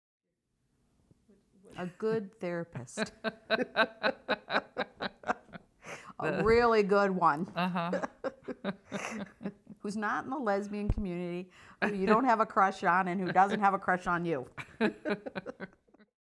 Not in My Lifetime: Baby Boomers on Marriage Equality
Phyllis Wiener and Shayna Berkowitz discuss the key to their 32-year relationship.  This is a short outtake from a new documentary by Pam Colby.  Now in production.